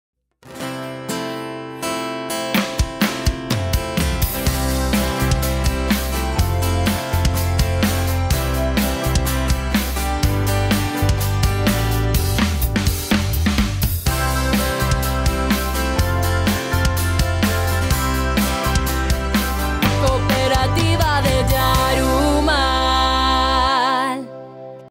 11.2 k Cooperativa de Yarumal
Te esperamos para vivir la gran experiencia que encierra la carrera atlética 11.2 k de la Cooperativa de Yarumal que este año llega renovada y cargada de nuevos premios y recorridos.

El reglamento al igual que el consentimiento informado y exoneración de responsabilidad civil, contractual y extracontractual en actividad peligrosa, lo puedes encontrar en nuestra pagina